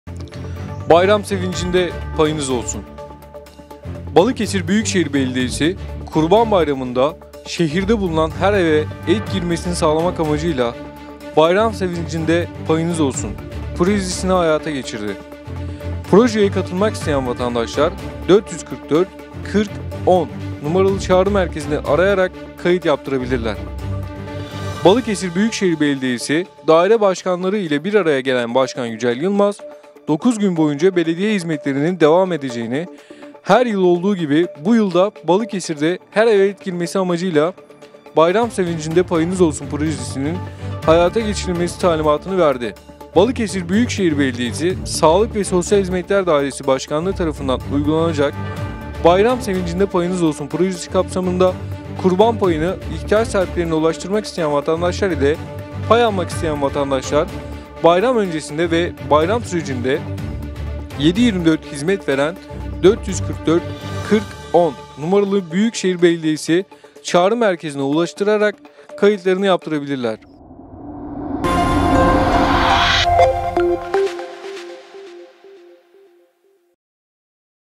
--Bayram sevincinde payınız olsun
Balıkesir Büyükşehir Belediyesi, Kurban Bayramı’nda şehirde bulunan her eve et girmesini sağlamak amacıyla “Bayram sevincinde payınız olsun” projesini hayata geçirdi.  Projeye katılmak isteyen vatandaşlar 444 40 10 numaralı çağrı merkezini arayarak kayıt yaptırabilirler.